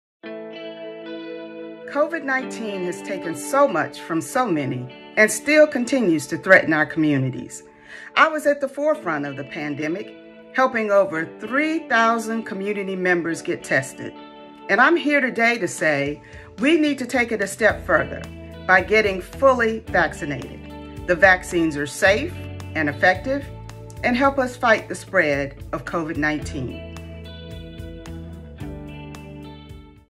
SCDHEC | COVID-19 Vaccine Influencers Representative Rosalyn Henderson Myers
"COVID-19 has taken so much from so many and still continues to threaten our communities." Rep. Rosalyn Henderson-Myers, SC House District 31, has been a COVID-19 community activist since the beginning of the pandemic, and is now encouraging South Carolinians to get vaccinated for COVID-19. Find a vaccine provider near you at scdhec.gov/vaxlocator.